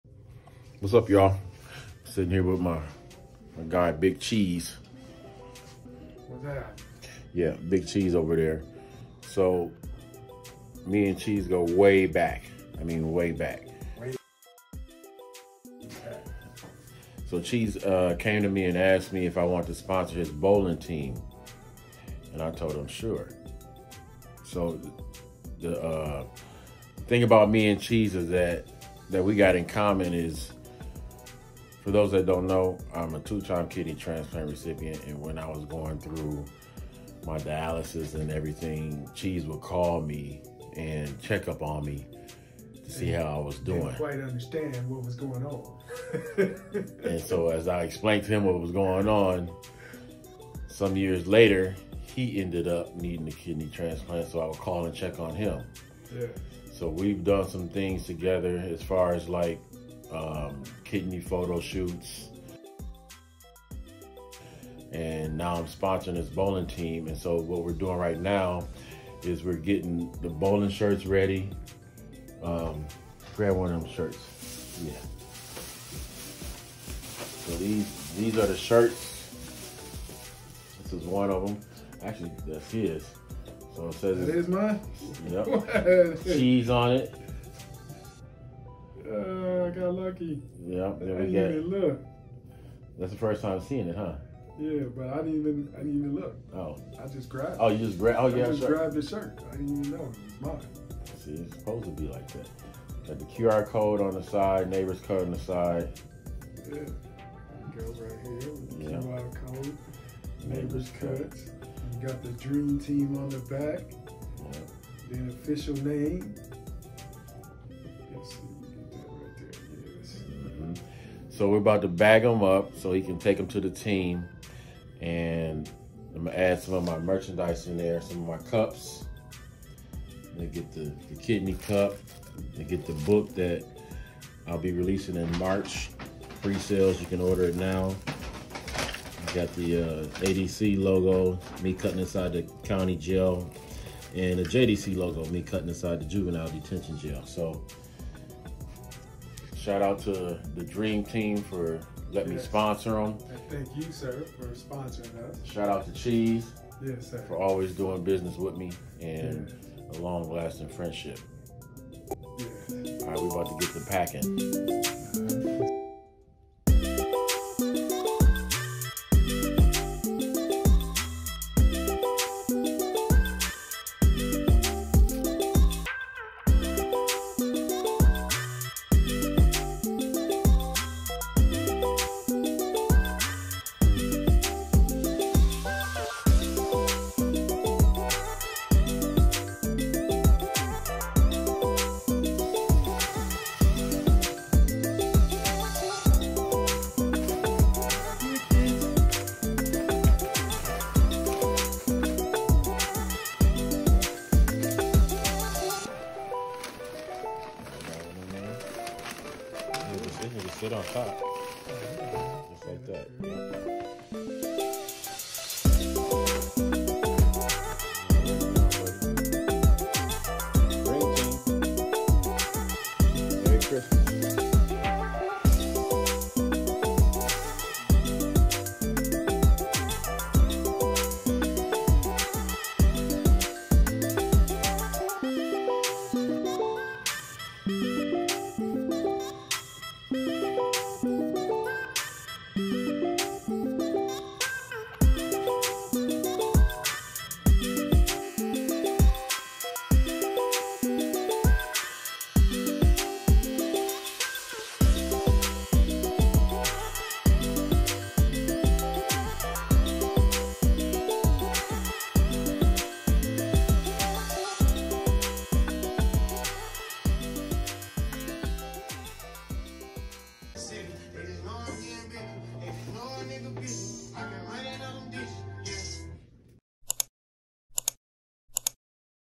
How a Friendship turned into Sponsorship…
Two friends that survived dialysis at different points in their lives have both received Kidney Transplants. To continue their friendship “Cheeze” asks “Robert” to sponsor his Bowling Team.